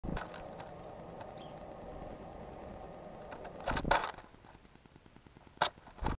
What Does a Black-capped Chickadee Look Like? 😄
Black-capped chickadee lands and takes sunflower seeds. A dog or cat might like watching this bird.